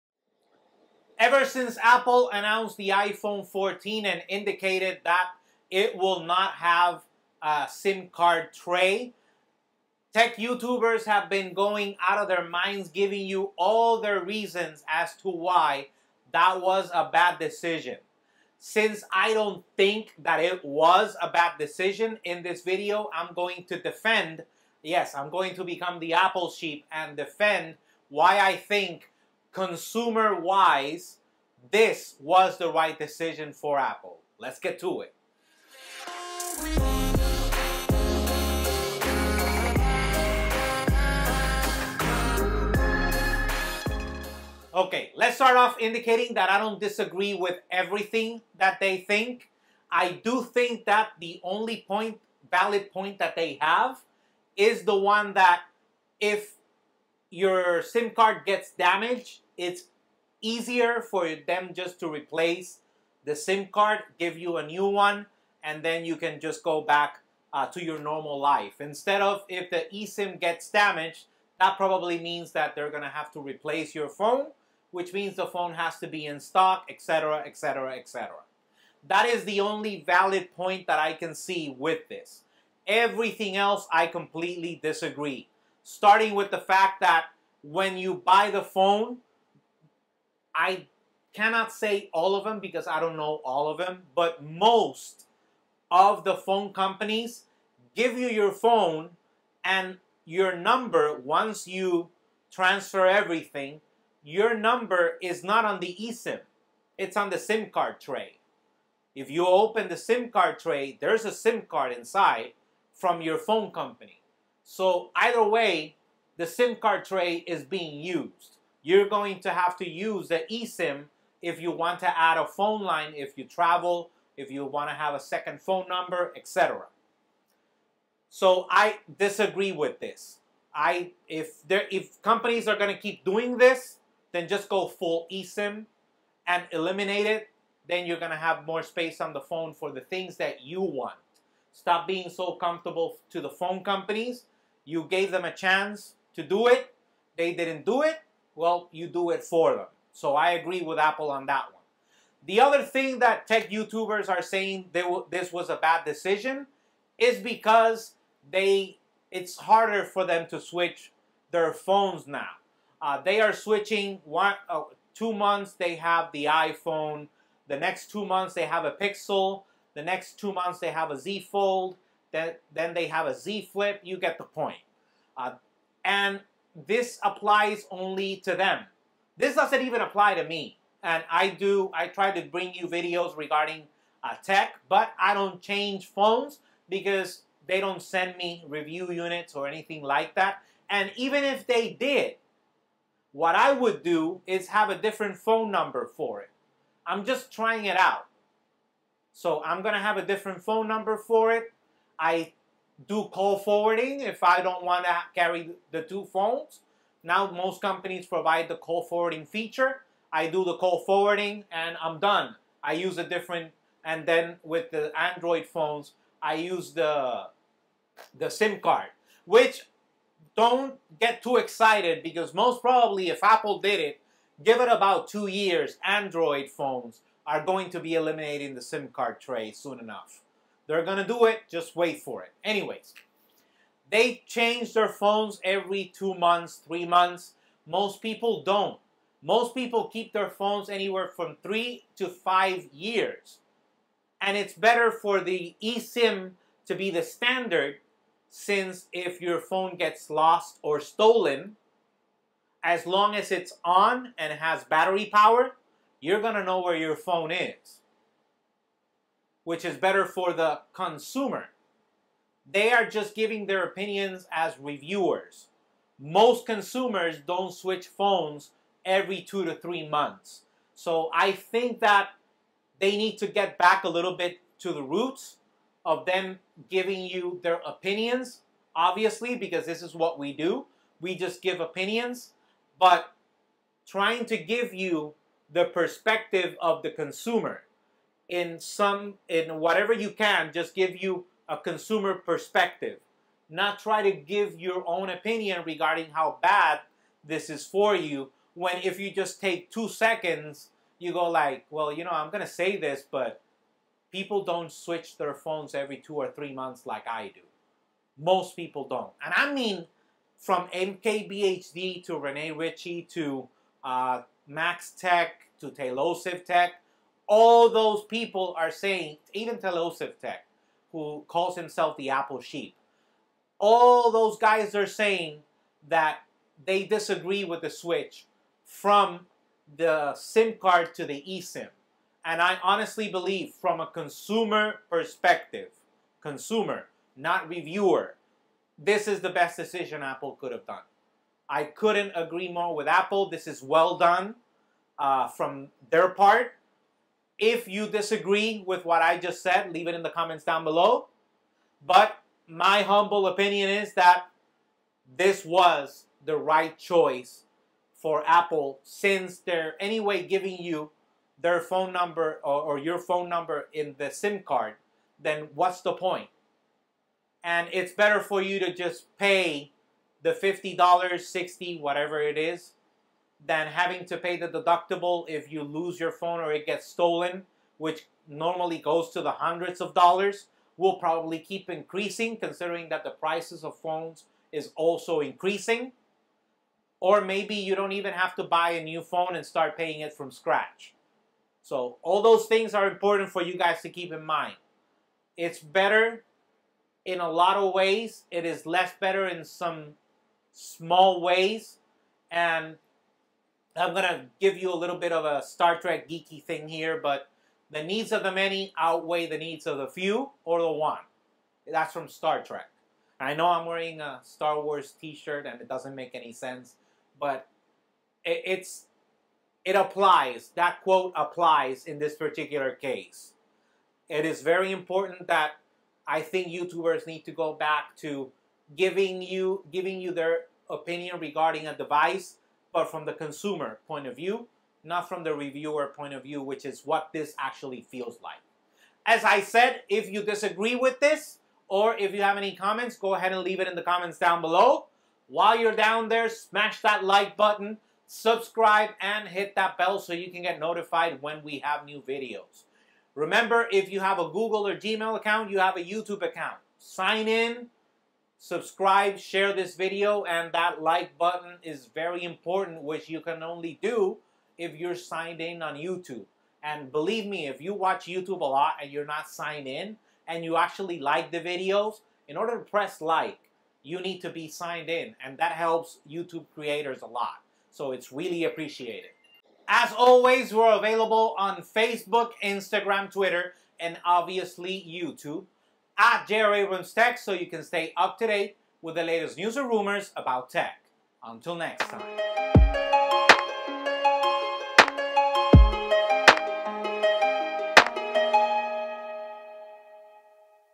My Opinion of Apple Switching from SIMCard to eSIM for the iPhone14 Pro
In this video I give you my opinion of Apple going full eSIM. Since I've heard YouTubers like @MaxTechOfficial @ReneRitchie @TailosiveTech @MKBHD and others saying this was a bad decision and my dissenting opinion since I've not seen anyone else defending it